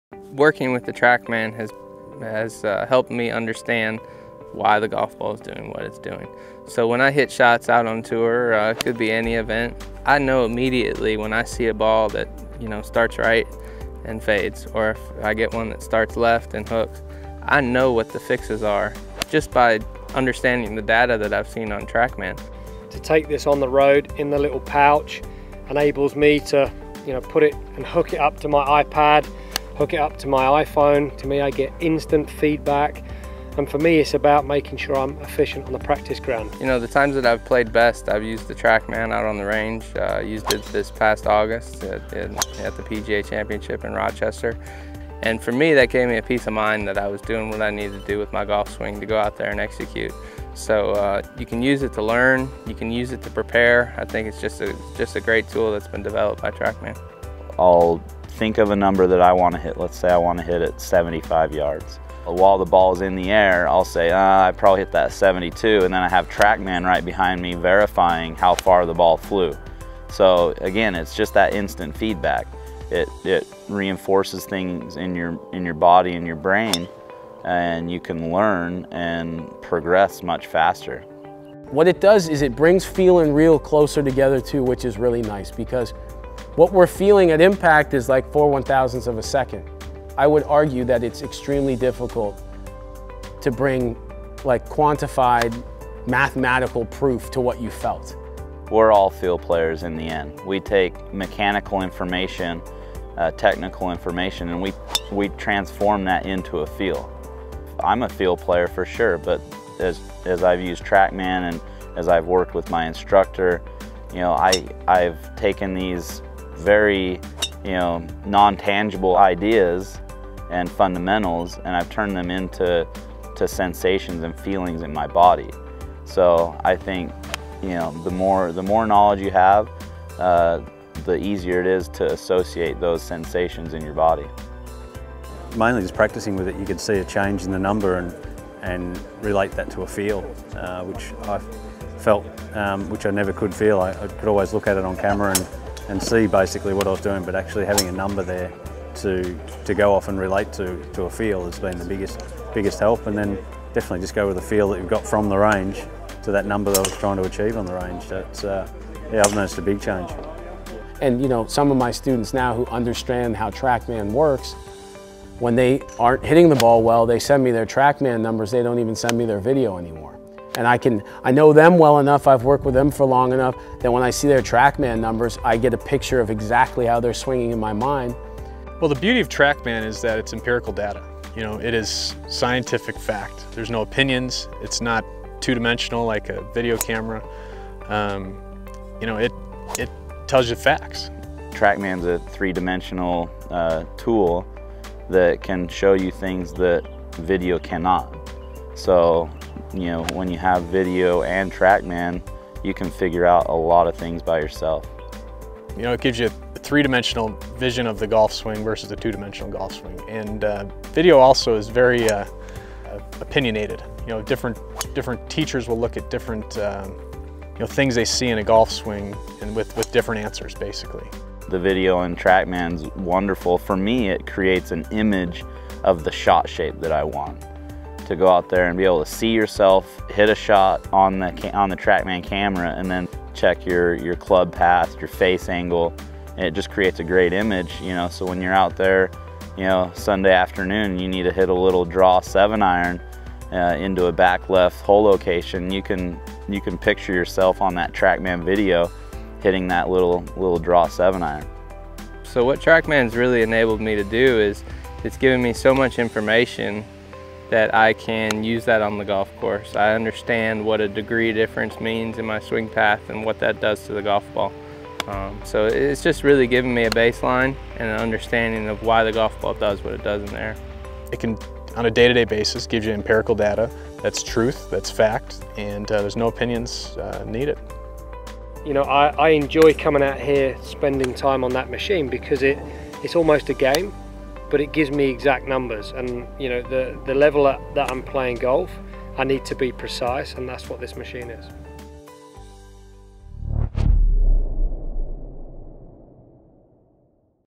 Top TOUR players on TrackMan
Listen to some of the worlds best TOUR players and Coaches discuss the benefits of using TrackMan.